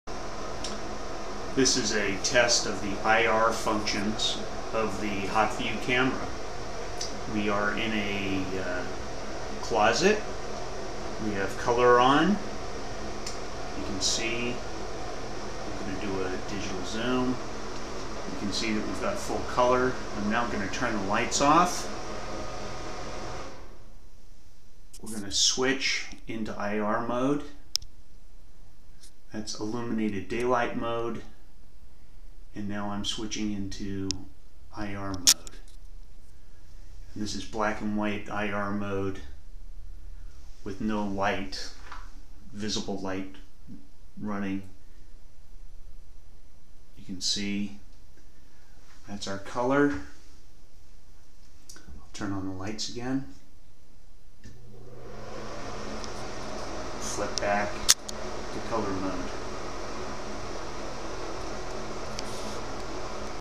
HauteVIEW 100 Infrared Performance
HauteSpot Networks HauteVIEW 100 Infrared camera performance. See the camera switch from visible to IR camera mode and back.